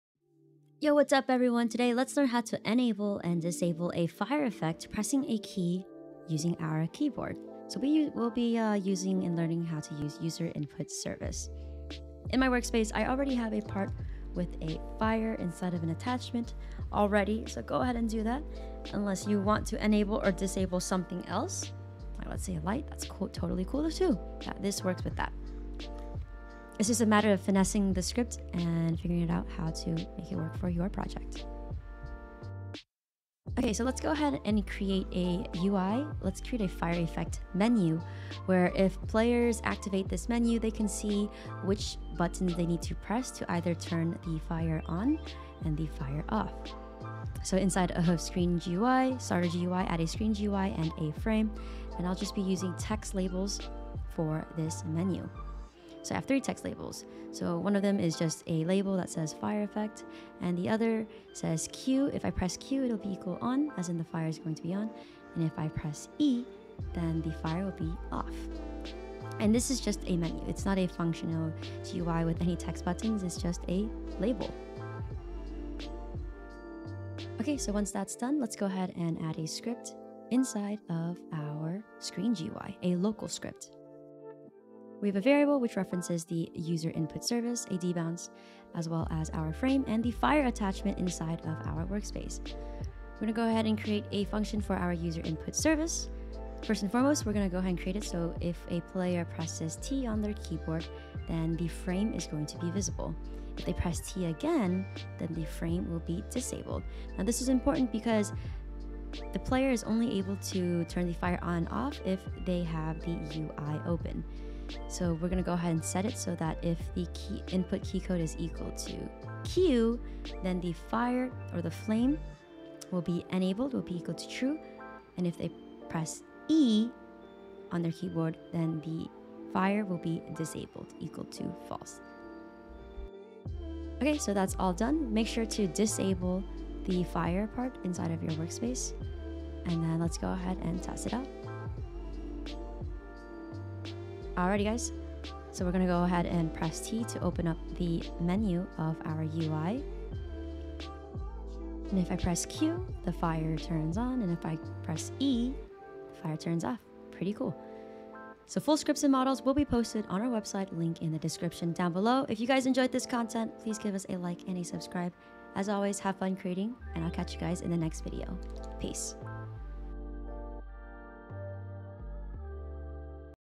How to Enable and Disable Fire Effect Presing a Key | User Input Service | Roblox Studio
Hiya developers! Watch this video to learn how to step by step enable and disable a fire effect when pressing a key on your keyboard for your game using Roblox Studio!